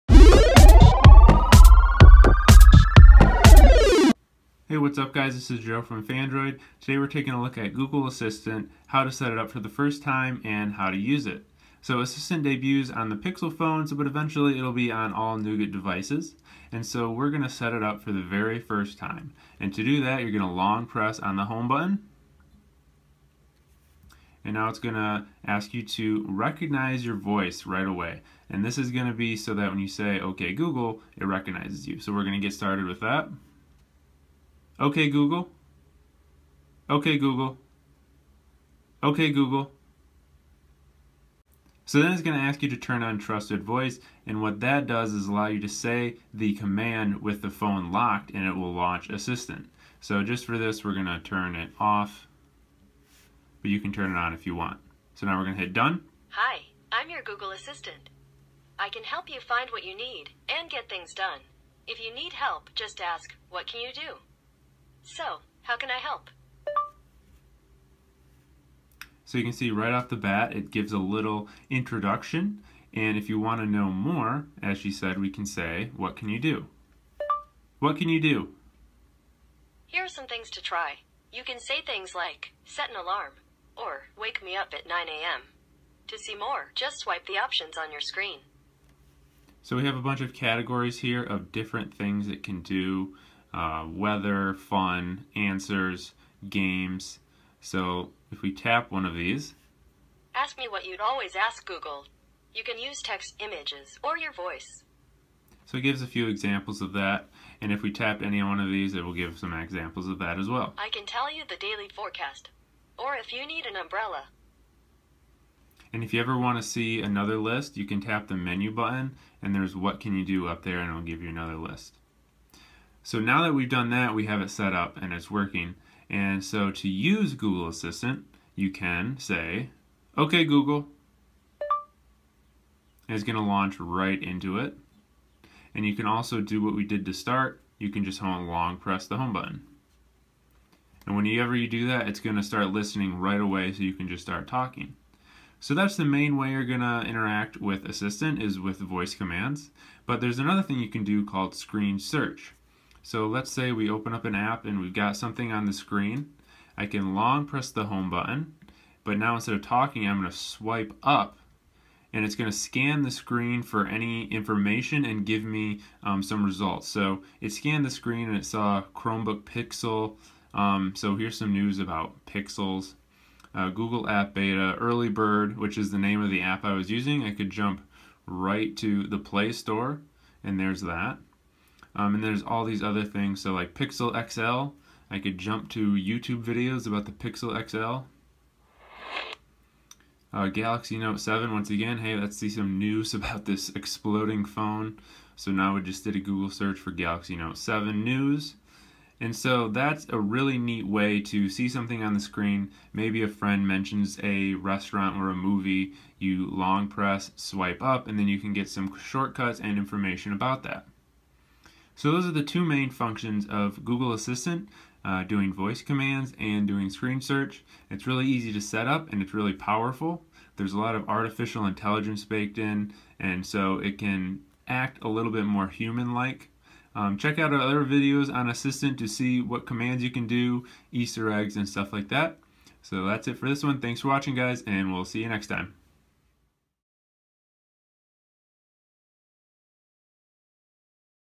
How to set up Google Assistant
Joe shows you how to set up and use Google Assistant. For more, visit phandroid.com